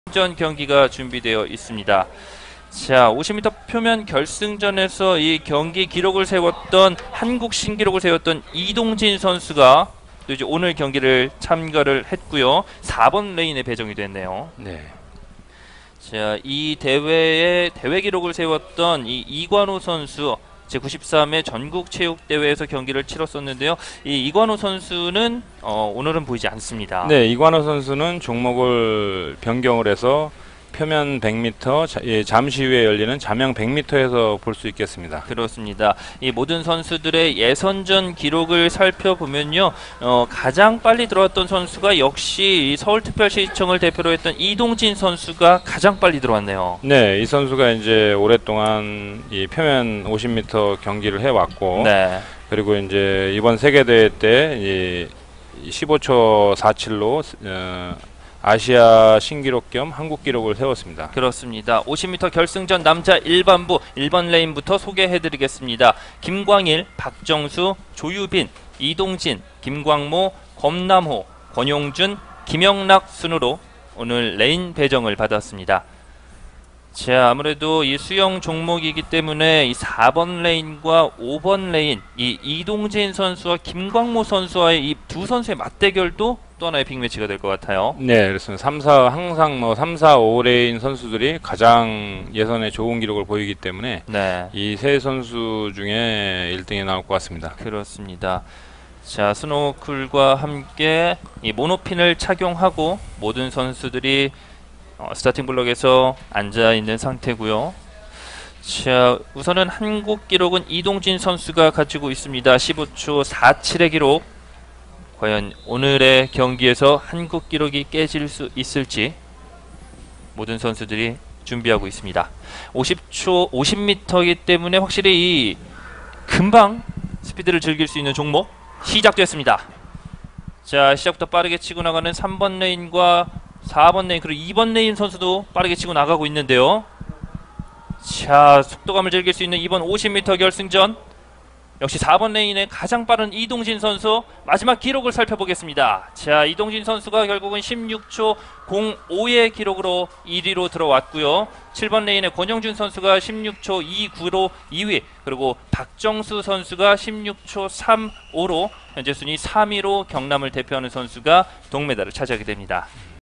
제96회전국체전/핀수영/남자일반부표면50결승
( 1위이동진 / 2위권용준 / 3위박정수 )